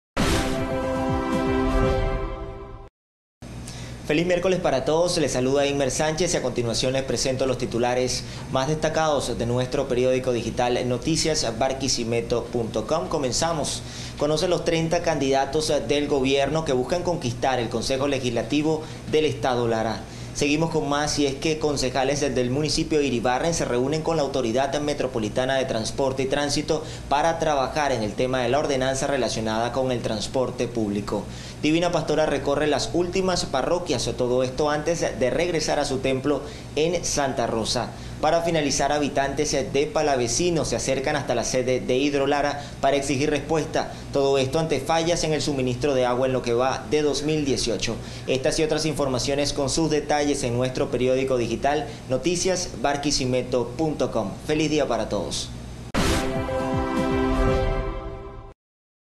Primer Resumen Informativo de Noticias Barquisimeto - Miércoles 21 de Marzo 2018
21 de Marzo | Recorre con nosotros las noticias regionales más destacadas del momento Barquisimeto Edo. Lara.